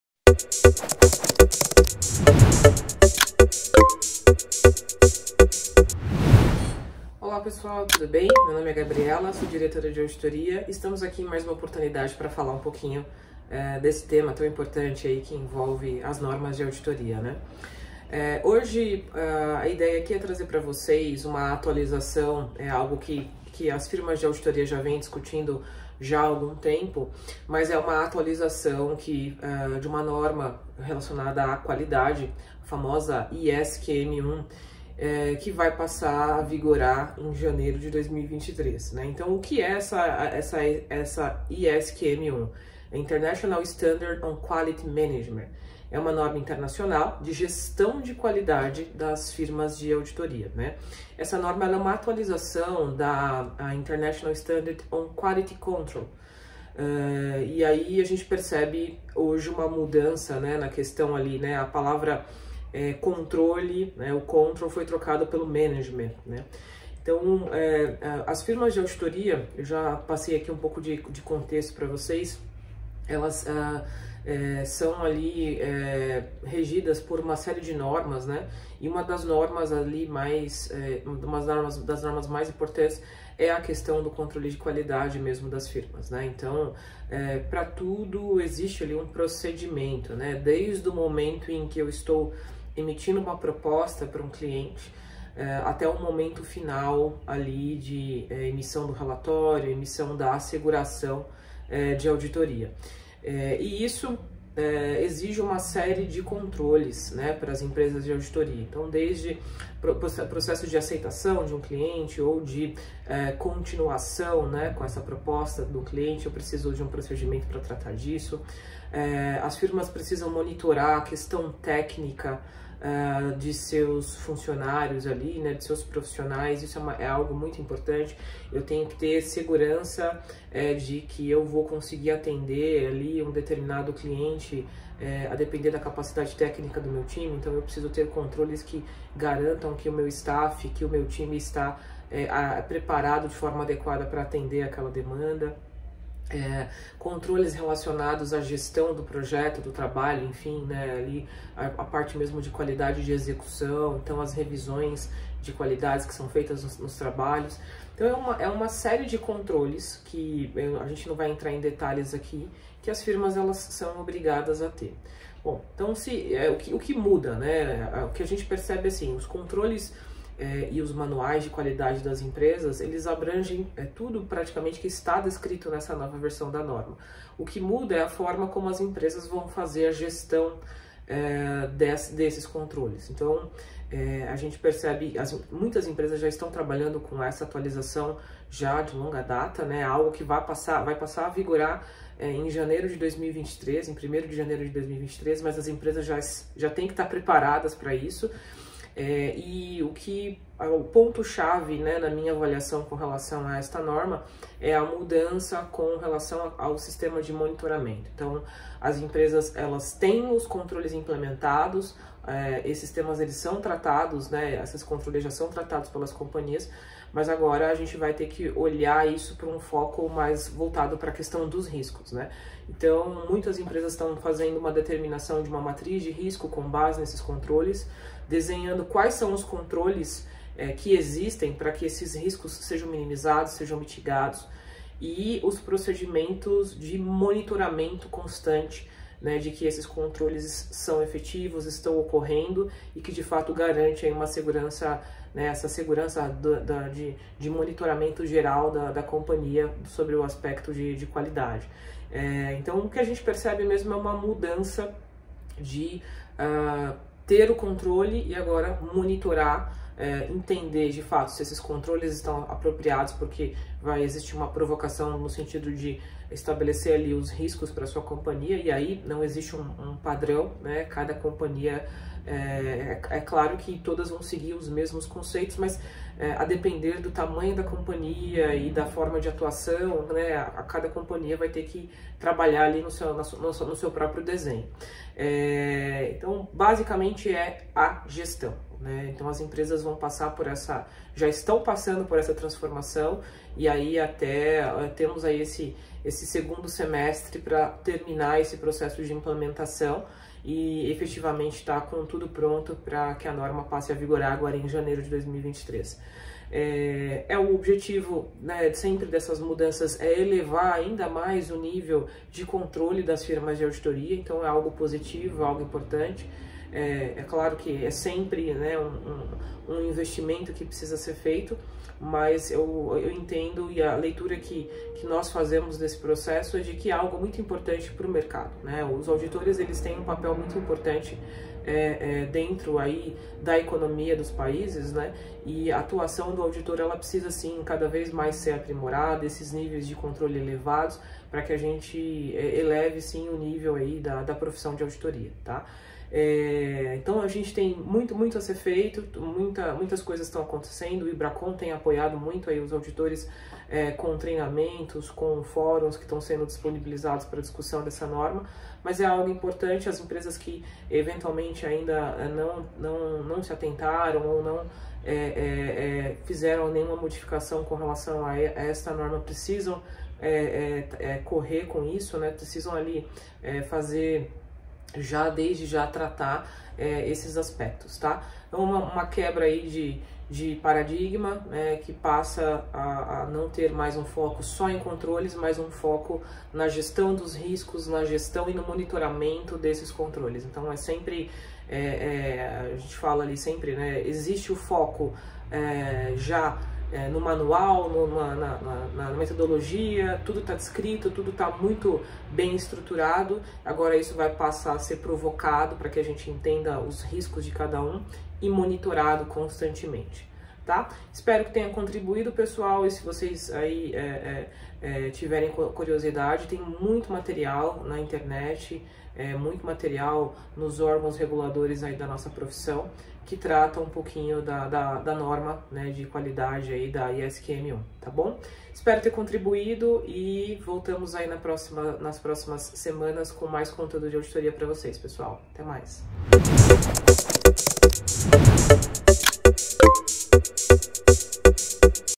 ISQM: Saiba tudo sobre a NOVA NORMA INTERNACIONAL SOBRE GESTÃO DA QUALIDADE ✔️
Assista ao vídeo e saiba tudo sobre a ISQM 1

O objetivo do ISQM 1 é fornecer segurança razoável para a empresa e garantir que seus funcionários conduzam os trabalhos com responsabilidade, de acordo com as normas e requisitos, assim como, emitir os relatórios adequados. No entanto, a garantia razoável é um nível de garantia alto, mas não absoluto.
Então, a ISQM 1 exige que uma empresa estabeleça um processo de monitoramento e remediação para fornecer informações relevantes, confiáveis e oportunas sobre o projeto, a implementação e a operação do sistema de gestão de qualidade, bem como tomar medidas para responder às deficiências identificadas. Nossa especialista Ana Gabriela explica como aplicar a ISQM e fornecer maior segurança dos processos de monitoramento.

🔴 ASSISTA TAMBÉM